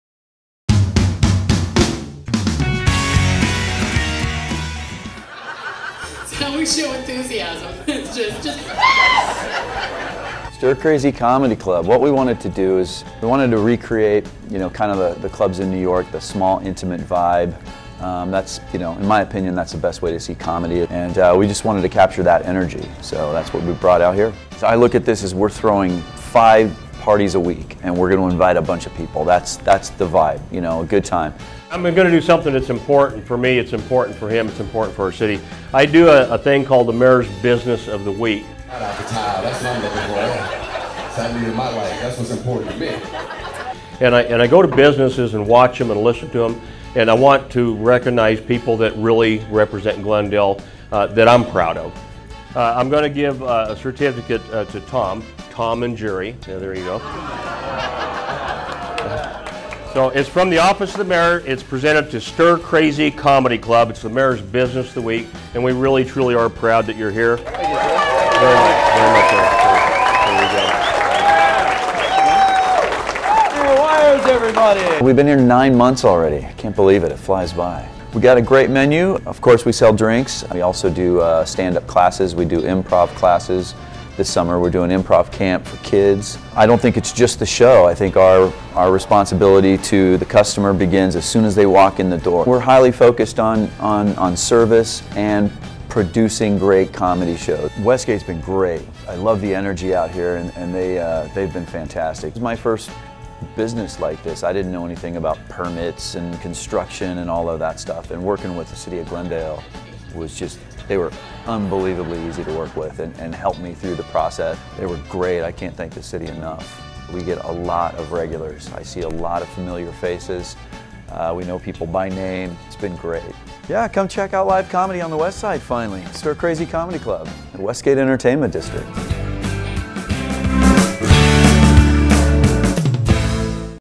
Stir Crazy Comedy Club Mayor's Business of the Week
Stir Crazy Comedy Club is Mayor of Glendale Jerry Weiers' Business of the Week